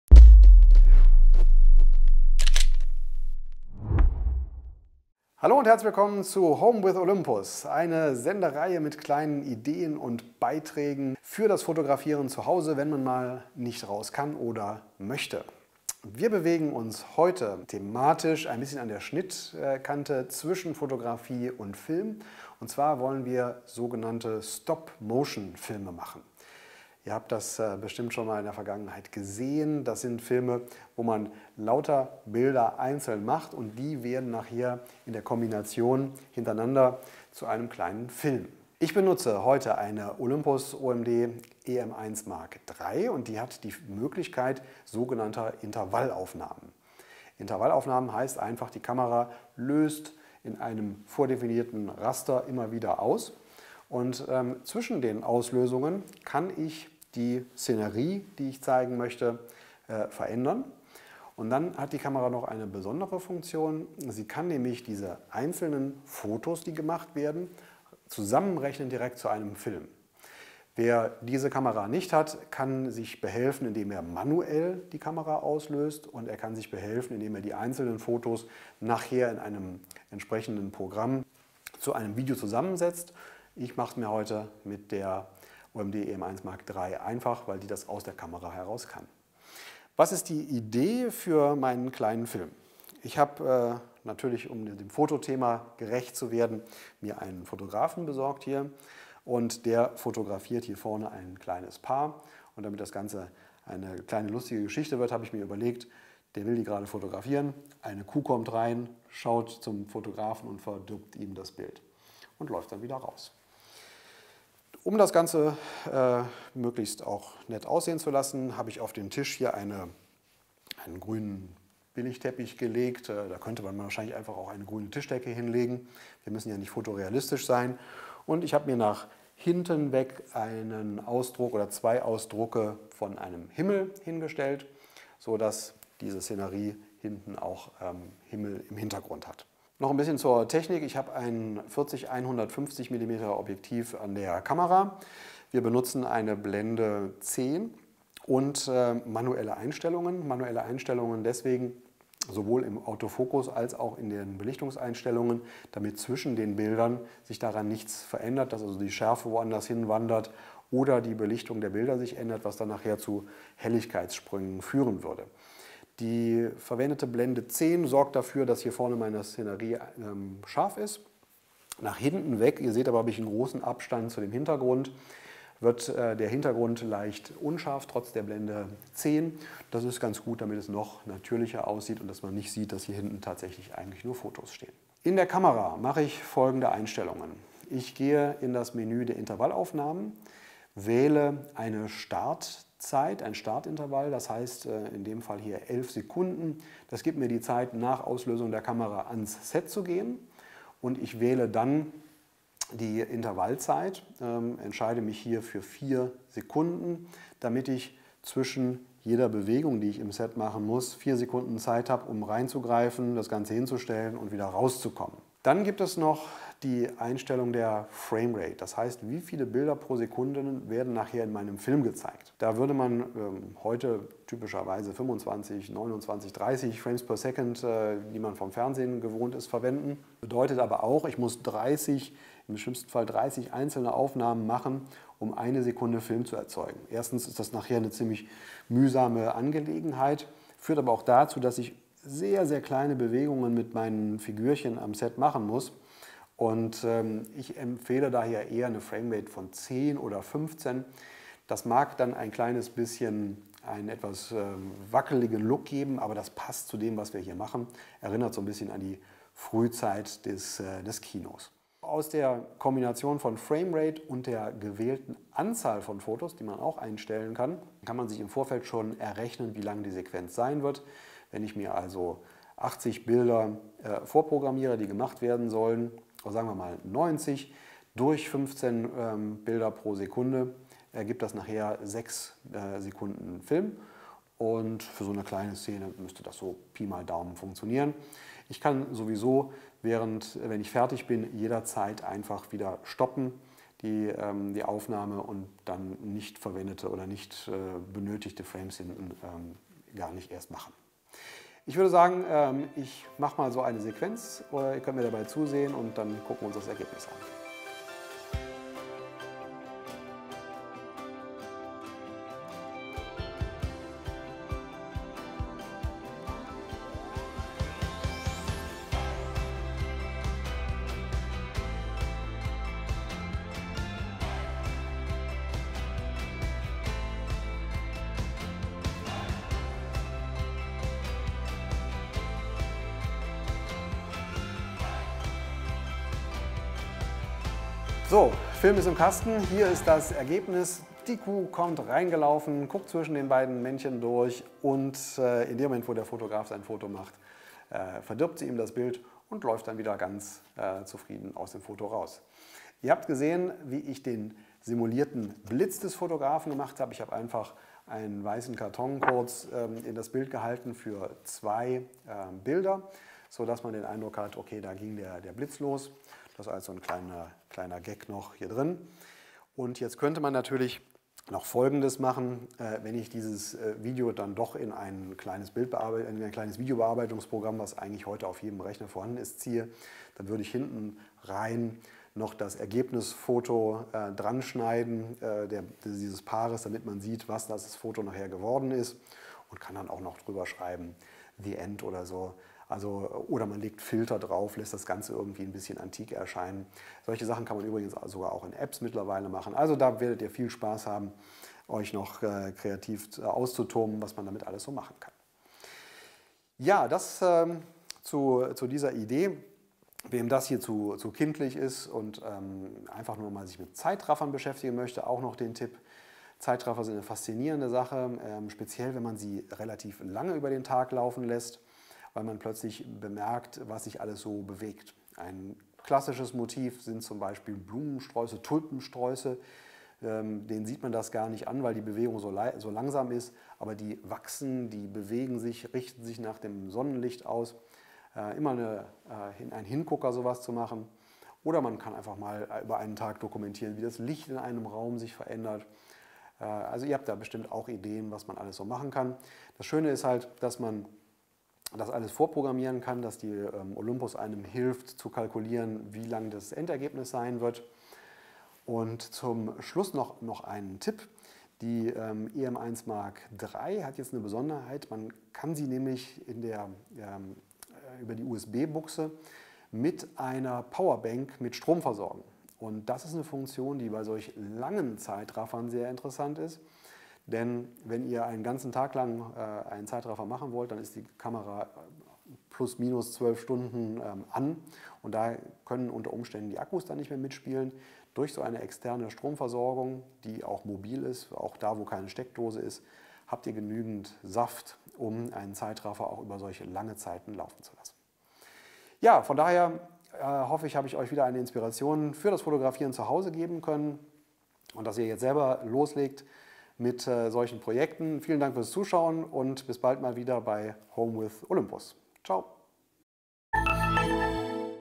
Home with Olympus | Tutorial Stop Motion | Wenn aus Fotos Filme werden
Stop Motion ist eine Filmtechnik, bei der einzelne Fotos zu einem Film zusammengefügt werden. In diesem Video erklärt Fotograf Marc Ludwig, wie Sie mithilfe von Intervallaufnahmen Ihren eigenen Stop Motion Film erstellen können und führt Sie durch die Kameraeinstellungen seiner Olympus OM-D E-M1 Mark III.